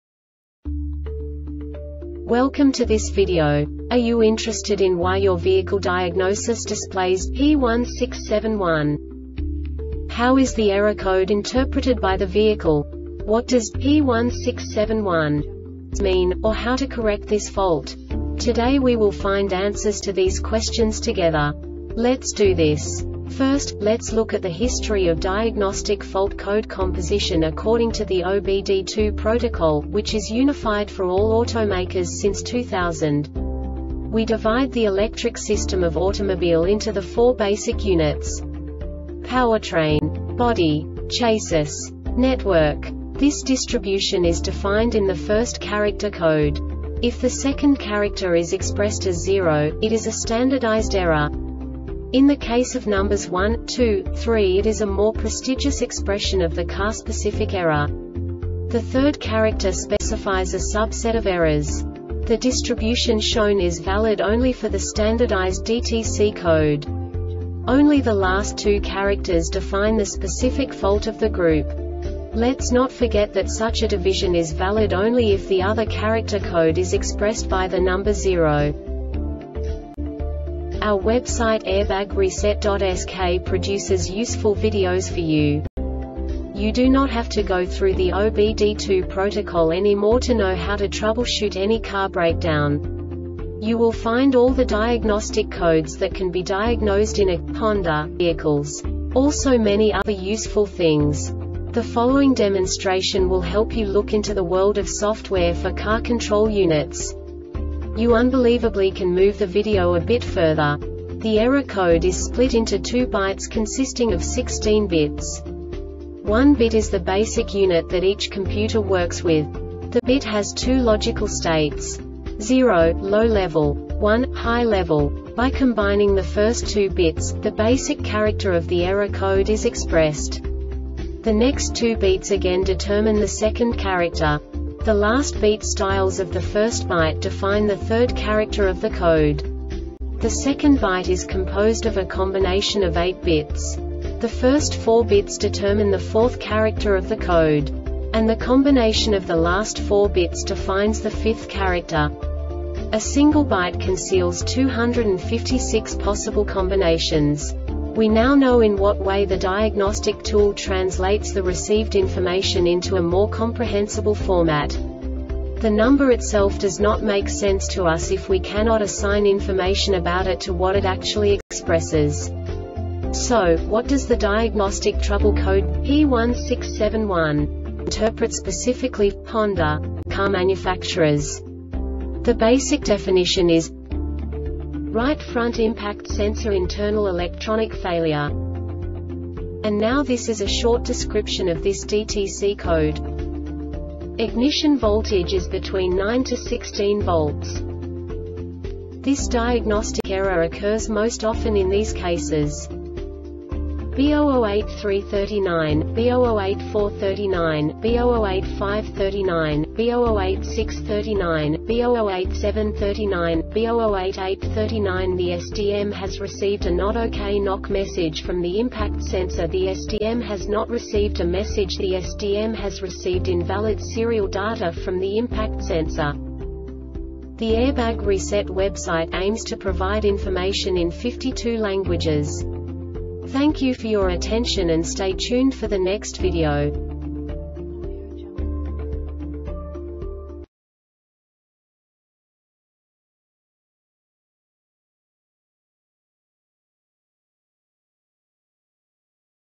DTC Honda P1671 Short Explanation
Contents:
0:21 Basic DTC analysis according to OBD2 protocol standard.
1:48 Insight into programming
2:58 A diagnostically understandable description of the Diagnostic Trouble Code
3:05 Basic error definition

Make:
Honda
Code:
P1671
Definition:
Right Front Impact Sensor Internal Electronic Failure
Description:
Ignition voltage is between 9-16 volts.
Cause:
B0083 39, B0084 39, B0085 39, B0086 39, B0087 39, B0088 39The SDM has received a Not Ok (NOK) message from the impact sensor.The SDM has not received a message.The SDM has received invalid serial data from the impact sensor.